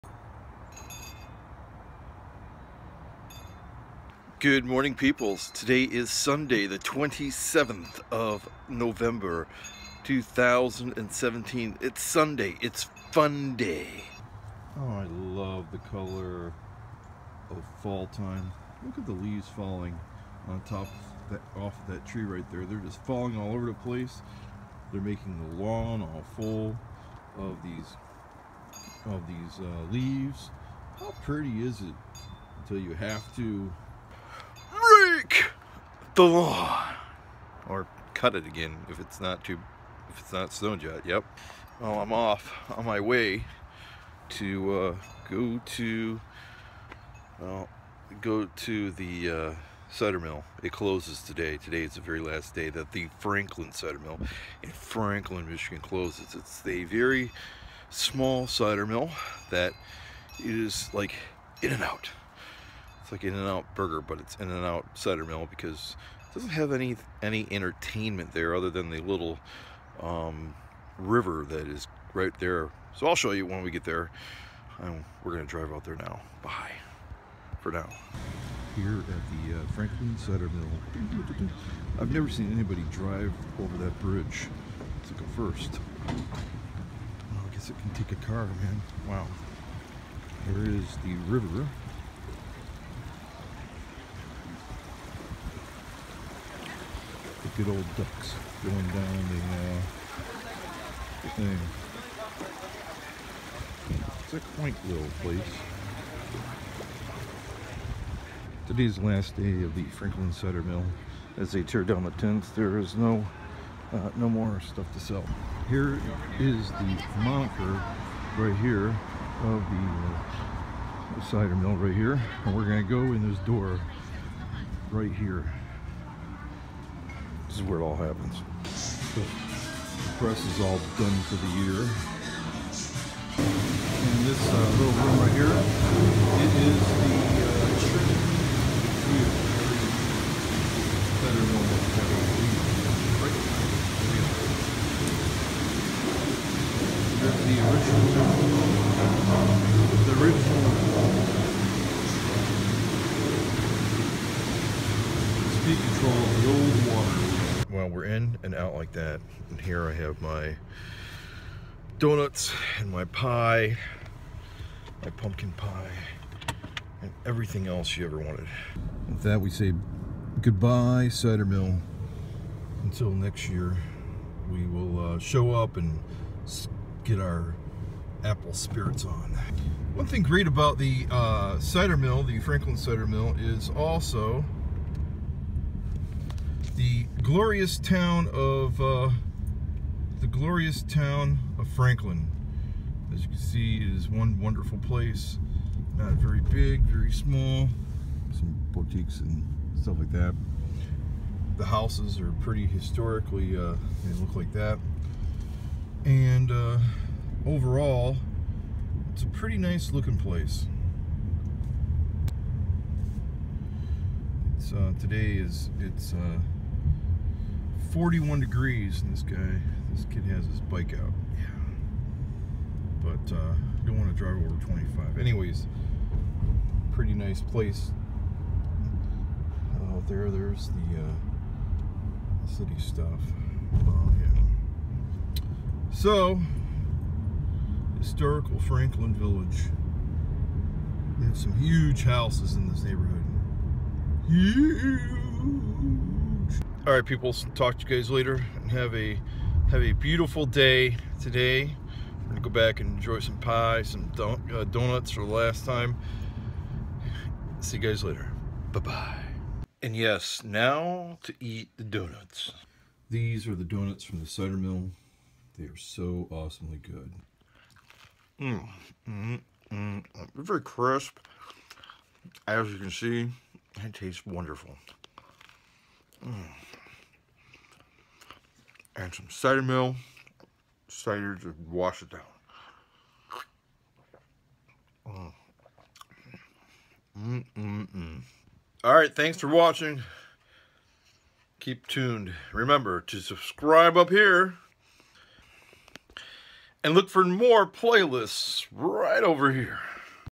Quick trip to the Franklin Cidermill, Franklin Michigan
Yummy Doughnuts and Cider. I love this place! :-)